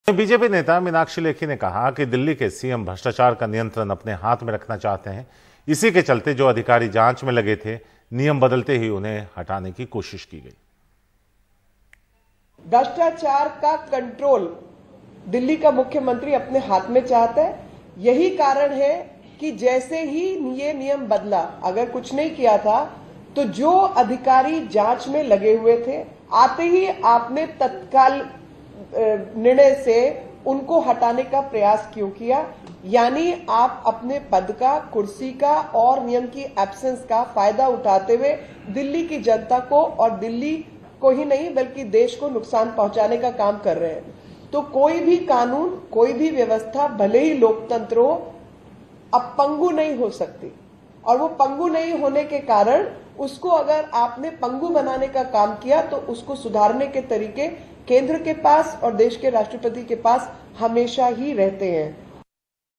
Arvind Kejriwal Delhi के भ्रष्टाचार का नियंत्रण अपने हाथ में रखना चाहते हैं : Meenakshi Lekhi
बीजेपी (BJP)  नेता मीनाक्षी लेखी (Meenakshi Lekhi) ने कहा कि अरविंद केजरीवाल (Arvind Kejriwal) दिल्ली (Delhi) के भ्रष्टाचार का नियंत्रण अपने हाथ में रखना चाहते हैं. इसी के चलते जो अधिकारी जांच में लगे थे, नियम बदलते ही उन्हें हटाने की कोशिश की गई. भ्रष्टाचार का कंट्रोल दिल्ली के मुख्यमंत्री अपने हाथ में चाहते हैं.